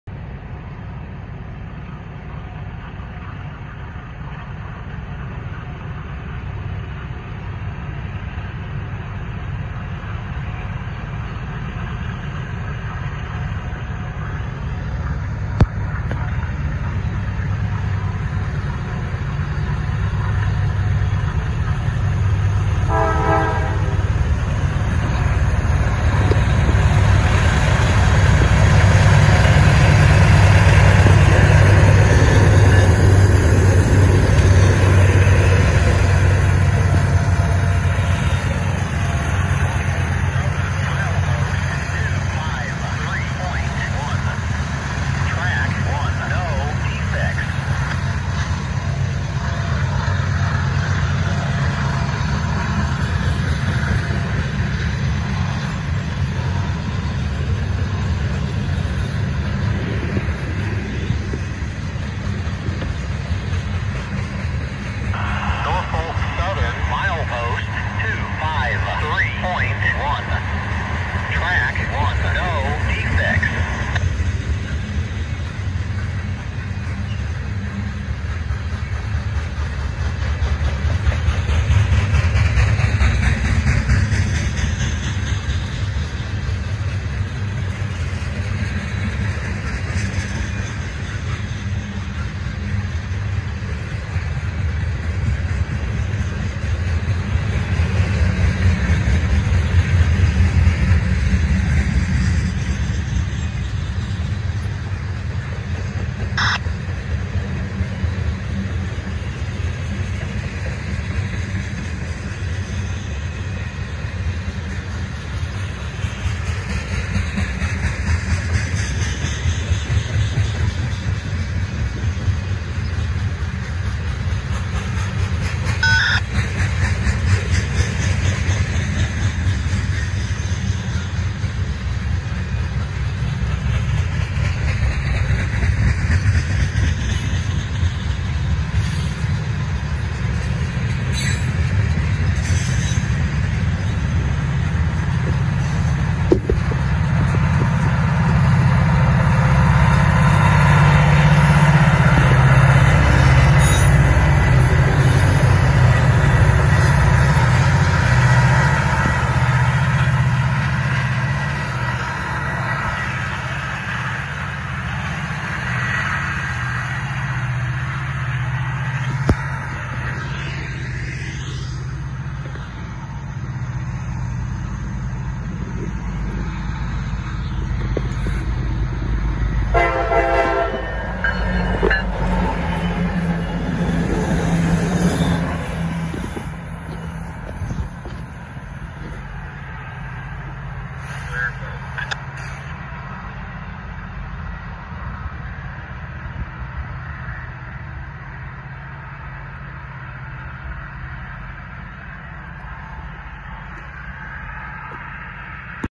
NS 6K4-04 with 9856 Leading with Bonus Helpers
6K4-04
NS 9856 - D9-44CW HH Built Mar 2004
NS 9686 - D9-44CW HH Built Mar 2001
NS 7337 - SD70ACU HH (Helper) Built Jan 2000 as CEFX SD9043MAC 135
NS 7339 - SD70ACU HH (Helper) Built Jul 1997 as UP SD9043MAC 8176

Helpers
NS 7237 - SD70ACU HH Built Jan 1996 as UP SD9043MAC 8007
NS 7243 - SD70ACU HH Built Jan 1996 as UP SD9043MAC 8013
NS 7247 - SD70ACU HH Built Feb 1996 as UP SD9043MAC 8018